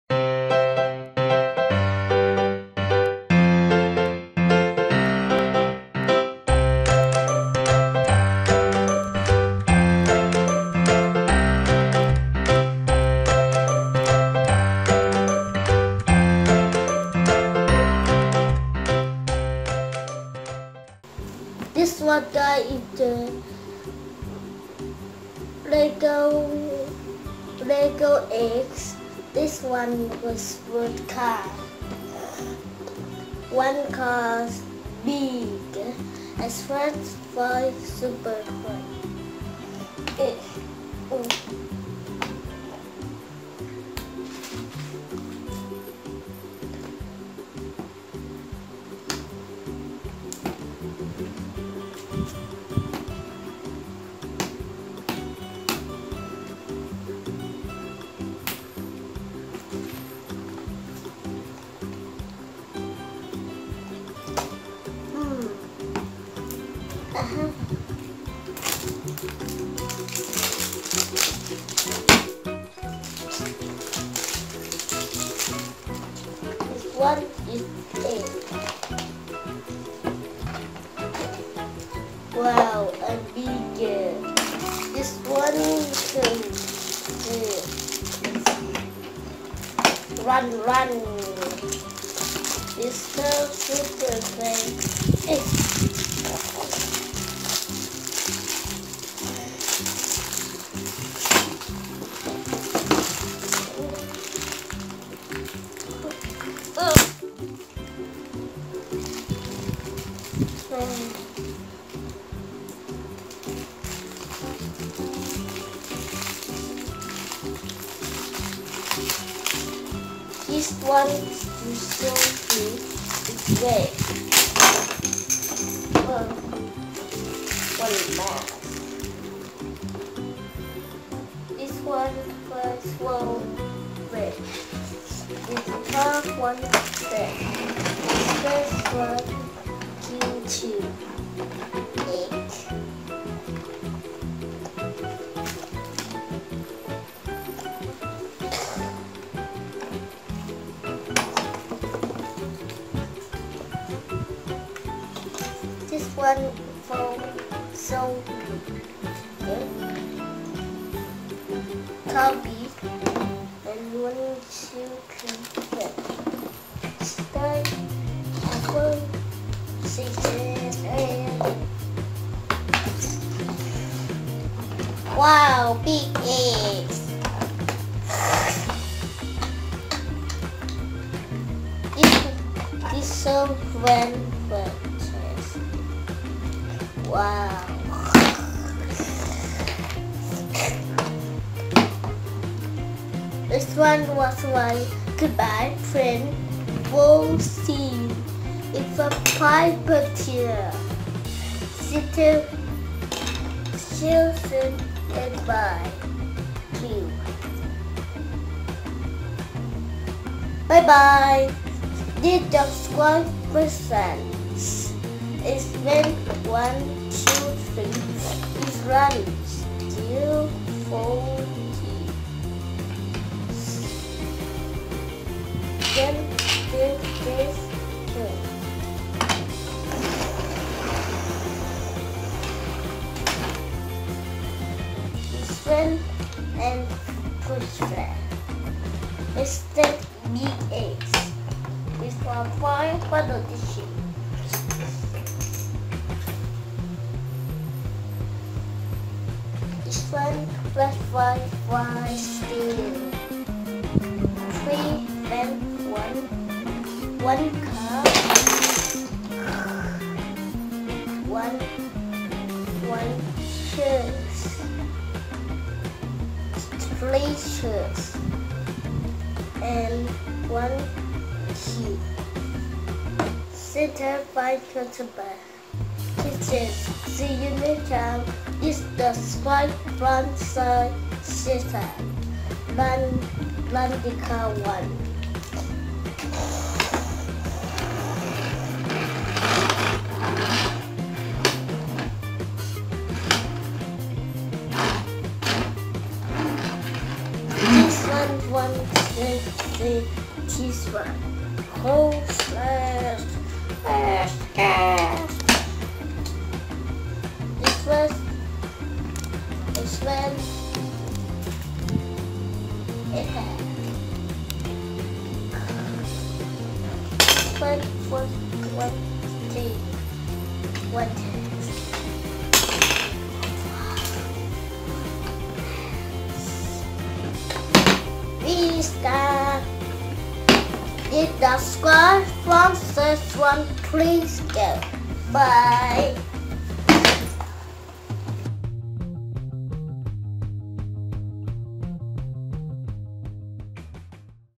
Egg Surprise Car Toy Giant Truck - YouTube Cheap Toys
Egg Surprise Car Toy Giant Truck present YouTube Cheap Toys. Cars Truck toys giant egg reviews the Car cartoon that Kid bought from 7-11 Thailand.